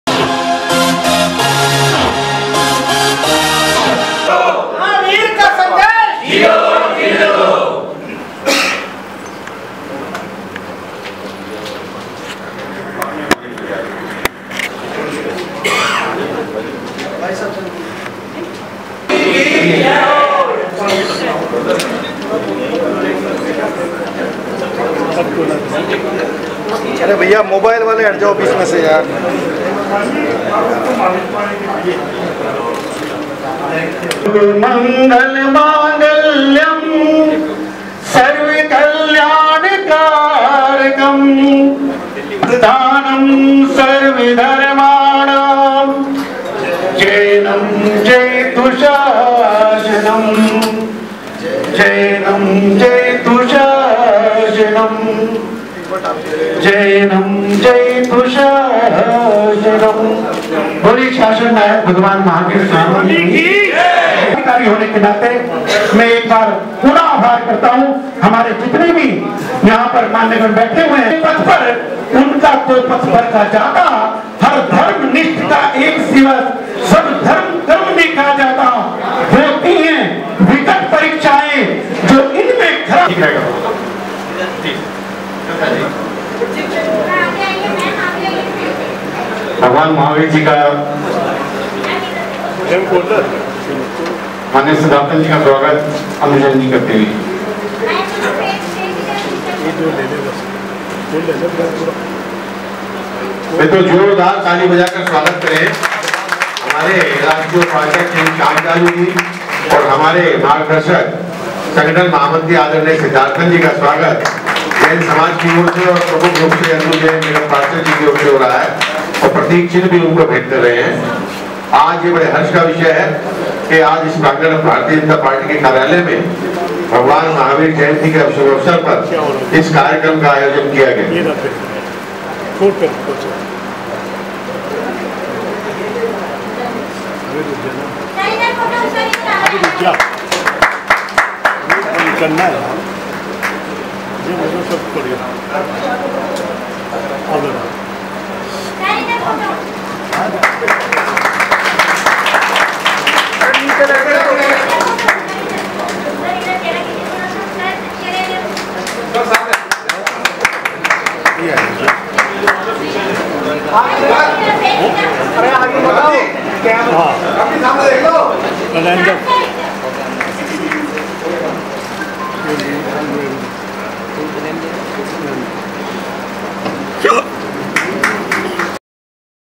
newsmakeinindia भाजपा ने मनाया महावीर जयंती कार्यक्रम
भाजपा के राष्ट्रीय उपाध्यक्ष श्याम जाजू को दिल्ली प्रदेश के संगठन महामंत्री  सिद्धार्थन जी की मौजूदगी में महावीर जयंती का कार्यक्रम आयोजित किया गया